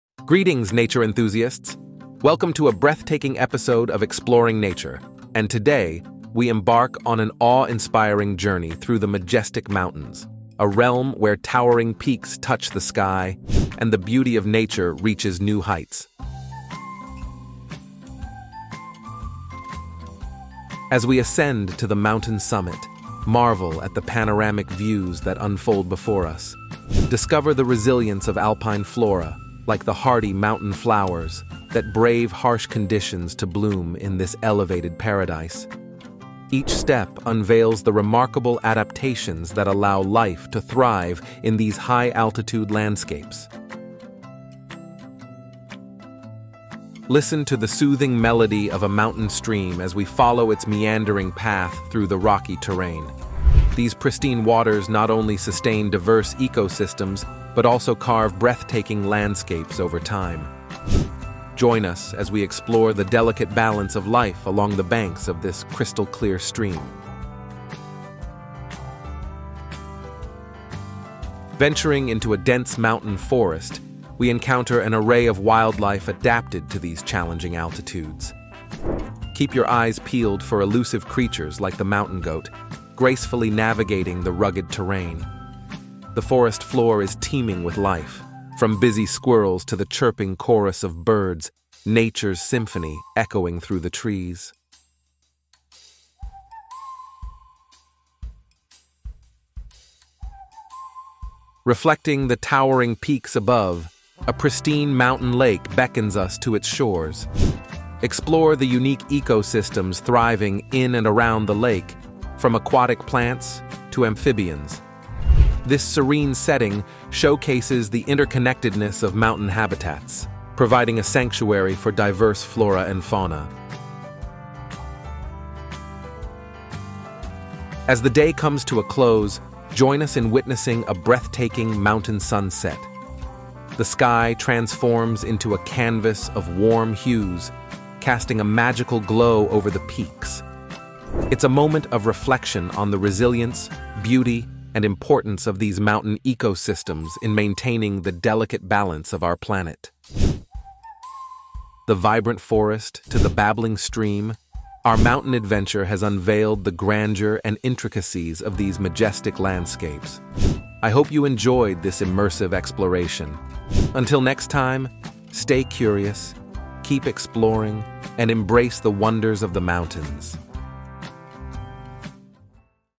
Exploring Nature: Peaks of Majesty
🏔 Embark on a Majestic Mountain Odyssey! 🌄✨

Join KidSmart on an awe-inspiring journey through the breathtaking landscapes of our latest episode, "Exploring Nature: Peaks of Majesty." Ascend to towering summits, trace the crystal-clear streams, and immerse yourself in dense mountain forests where diverse wildlife thrives. From serene lakes reflecting the peaks to the magical hues of a mountain sunset, discover the grandeur and intricacies of these majestic landscapes. Ready to be captivated by the beauty of nature's masterpiece? Let's explore the Peaks of Majesty! 🌲🦌 MountainAdventure ExploringNature NatureInspiration 🌿